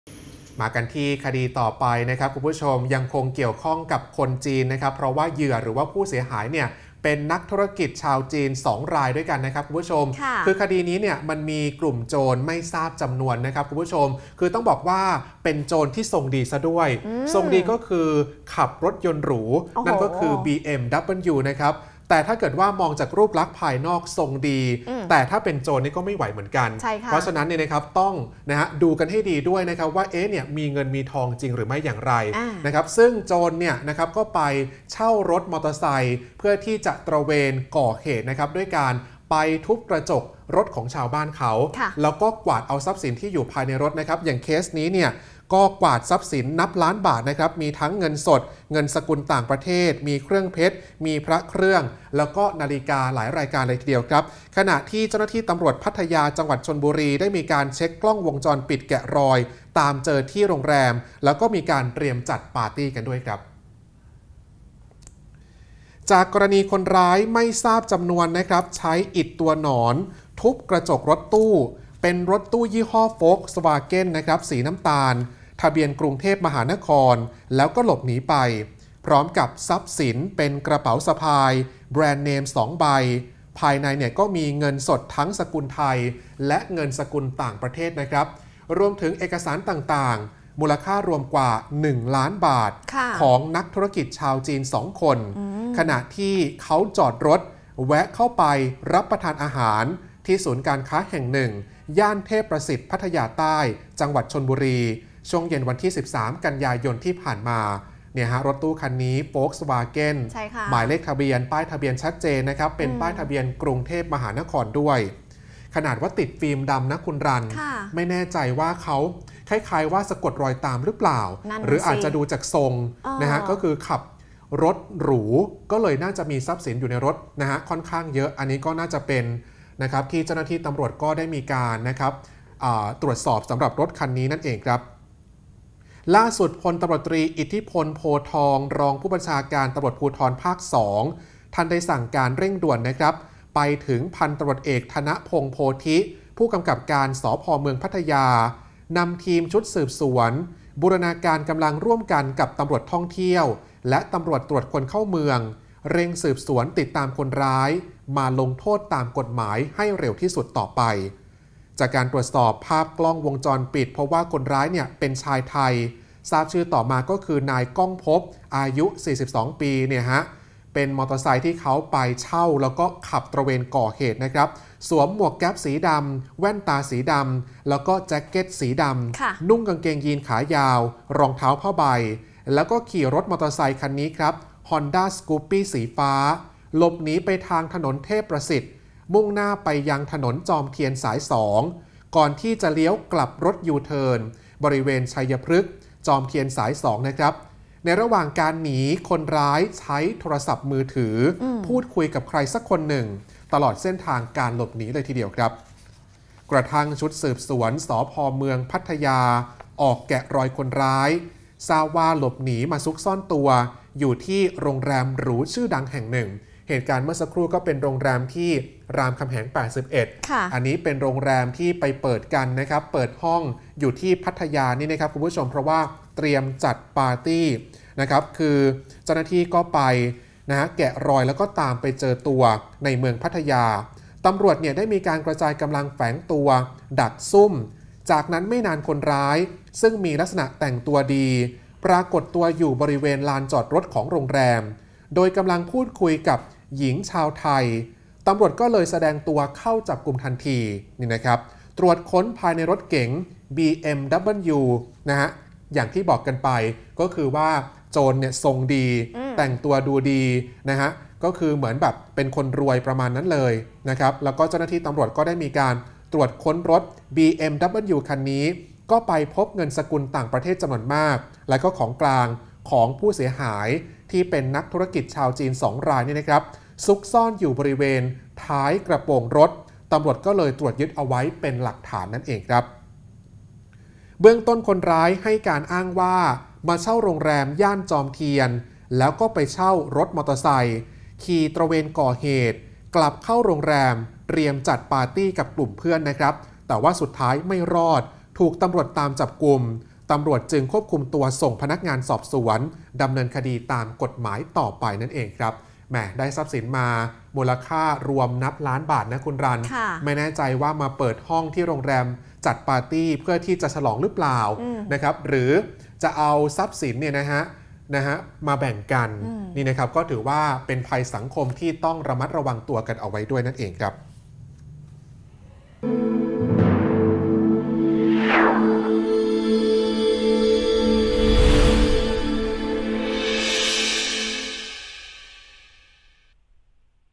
ตร.พัทยาจับโจรเช่าจยย.ตระเวนทุบกระจกรถ กวาดทรัพย์ล้านบาท | รอบวันทันเหตุการณ์ 15 ก.ย.66
โจรหนุ่มทรงดี ขับรถเก๋ง BMW เช่ารถ จยย.คว้าอิฐทุบกระจกรถเหยื่อสองนักธุรกิจชาวจีน กวาดทรัพย์นับล้านบาท ตร.พัทยา เช็คกล้องวงจรปิดแกะรอย ตามเจอที่โรงแรมเตรียมจัดปาร์ตี้ สุดท้ายไม่รอดถูกรวบพร้อมของกลางเงินสด เครื่องเพชร พระเครื่อง นาฬิกา หลายรายการ